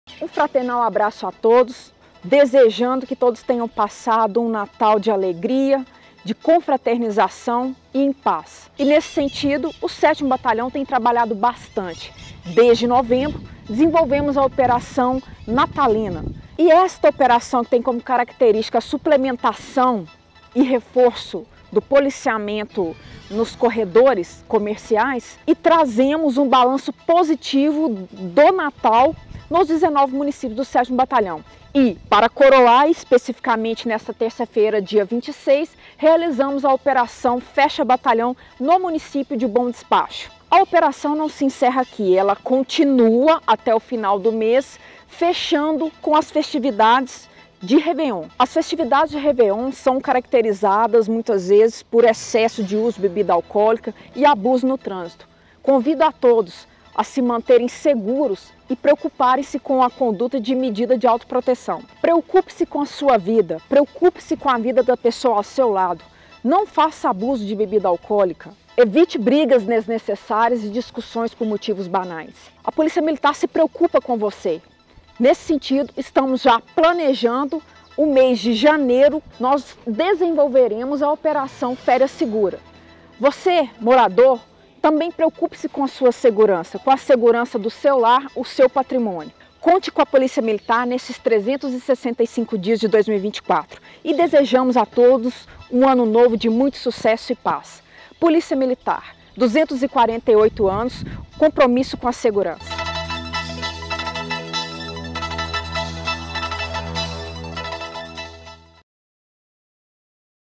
7° Batalhão apresenta o balanço do Natal 2023, expectativa Reveillon e Operação Férias Seguras
Divulgue sua marca em nossos vídeos: (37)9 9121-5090